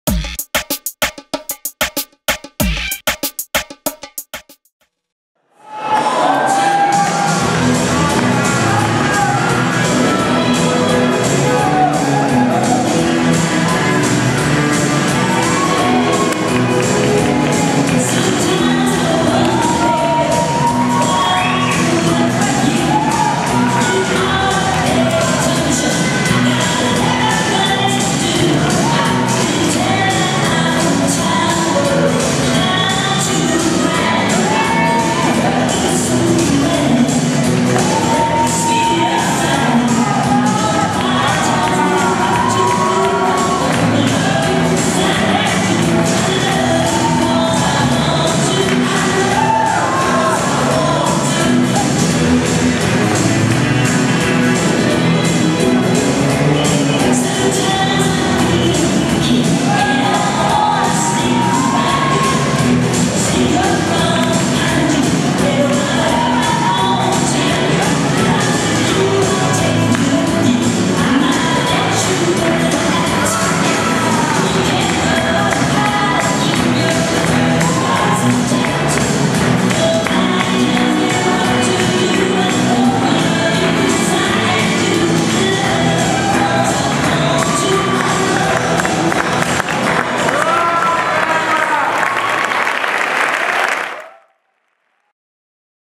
Timur Imametdinov - Ekaterina Nikolaeva, GOC 2010 Stuttgart, IDSF grand slam latin, 4.round - jive
Timur Imametdinov - Ekaterina Nikolaeva (Russia)
Имаметдинов Тимур - Николаева Екатерина (Россия)
German Open Championships 2010, Stuttgart (GOC)
IDSF grand slam latin
4. round
jive